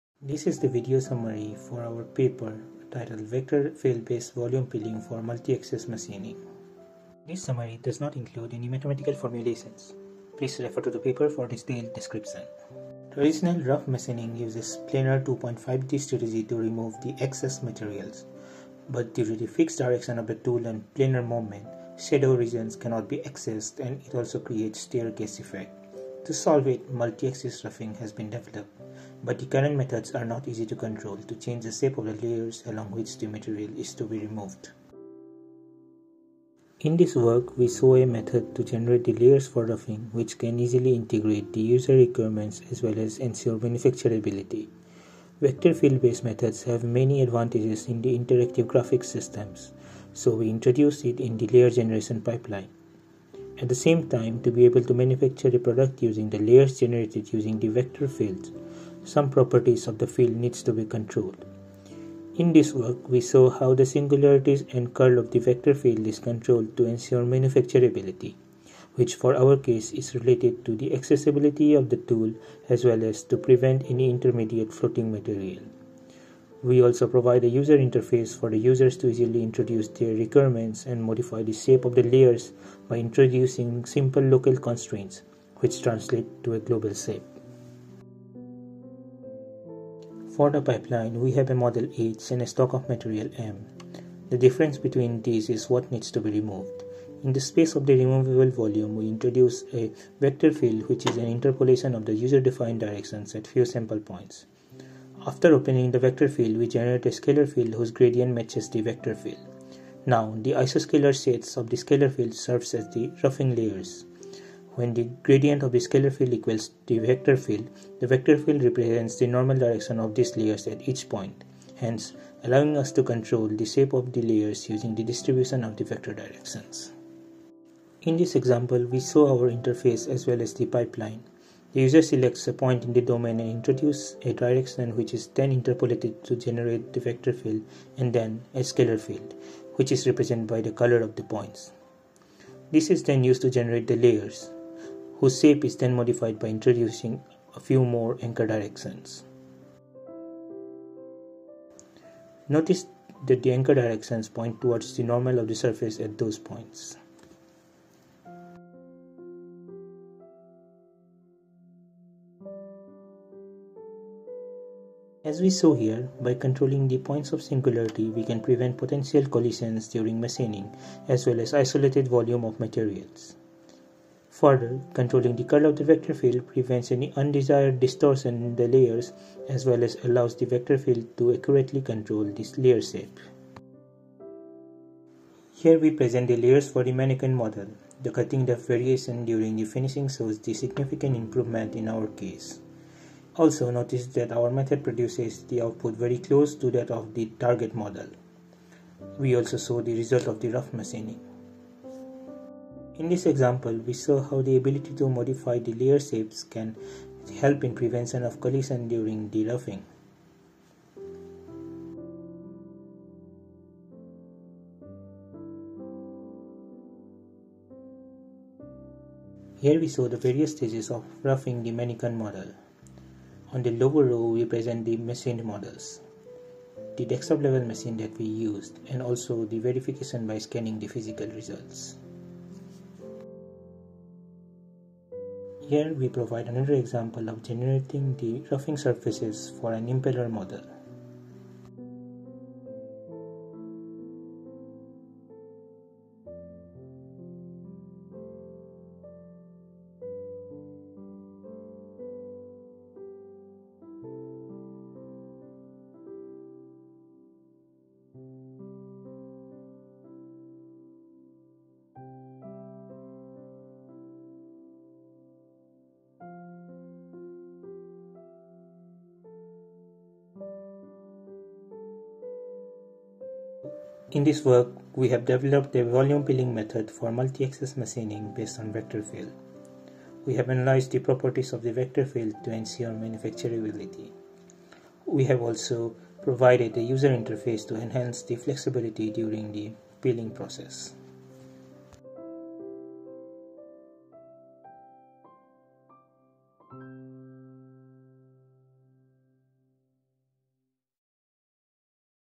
Vector Field Based Volume Peeling for Multi-Axis Machining
This video demonstrates our research work presented in the following paper:

Neelotpal Dutta, Tianyu Zhang, Guoxin Fang, Ismail E. Yigit, and Charlie C.L. Wang, "Vector Field Based Volume Peeling for Multi-Axis Machining", under review.